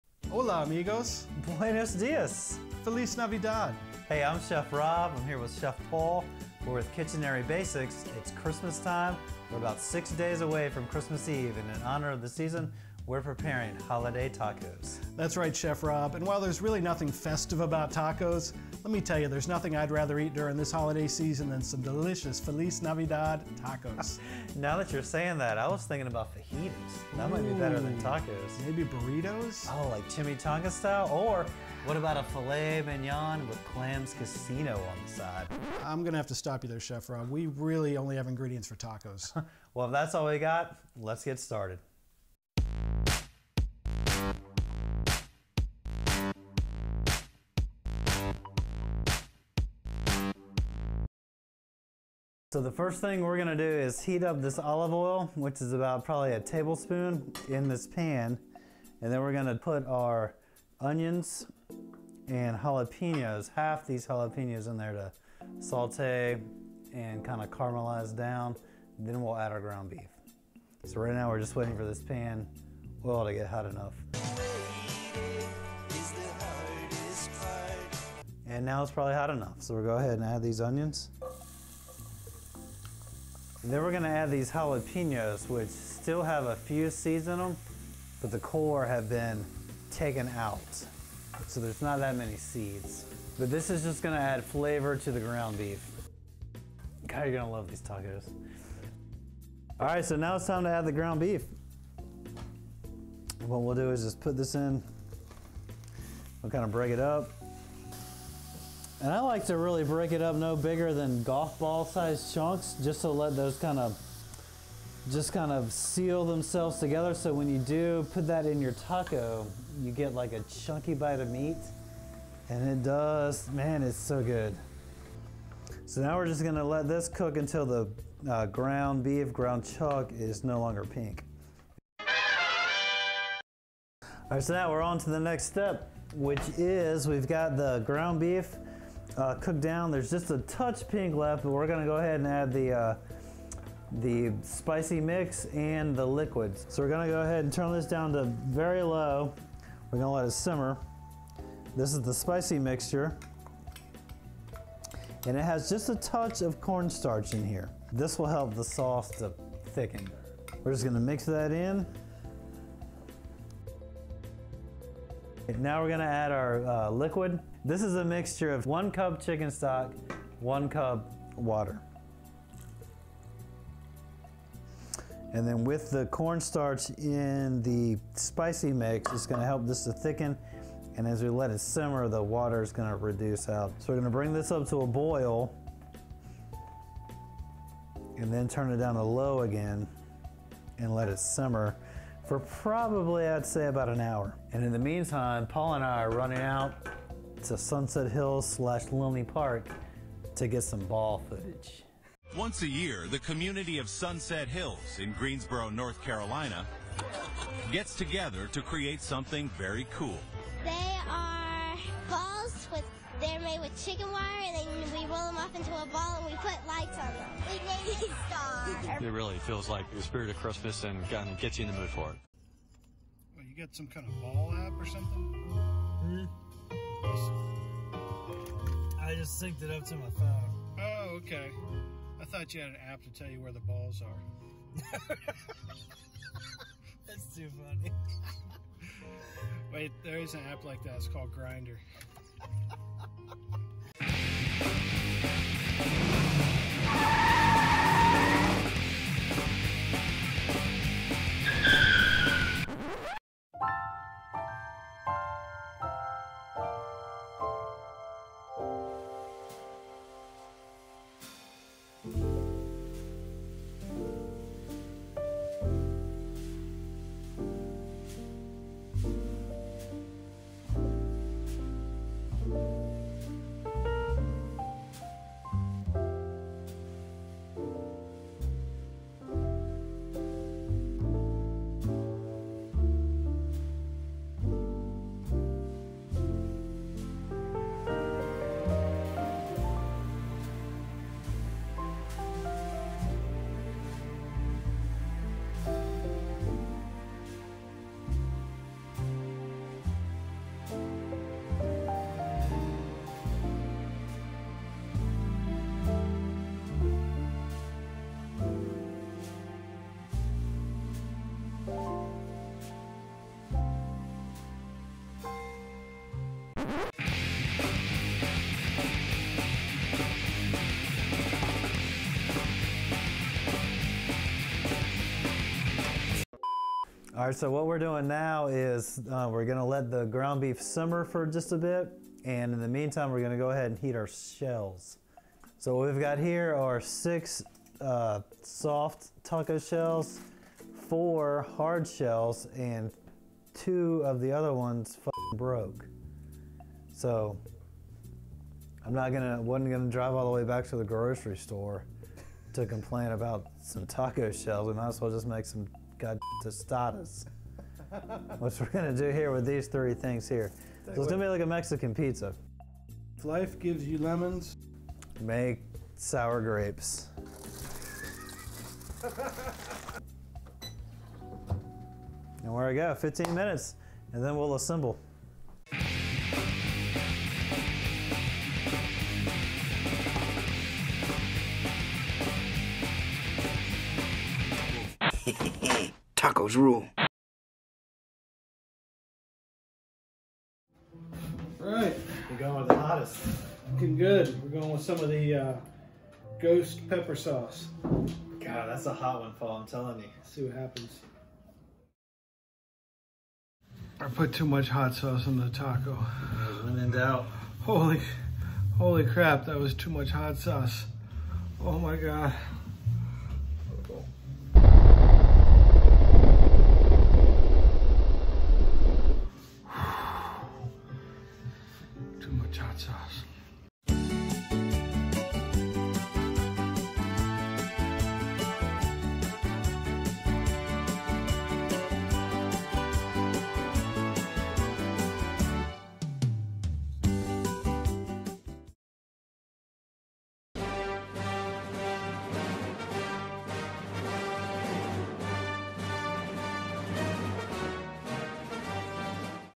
Kitchenary Basics | Christmas Light Balls & TACOS!
Revel in the beauty of Greensboro NC's Christmas light ball display and learn to make the ultimate holiday taco!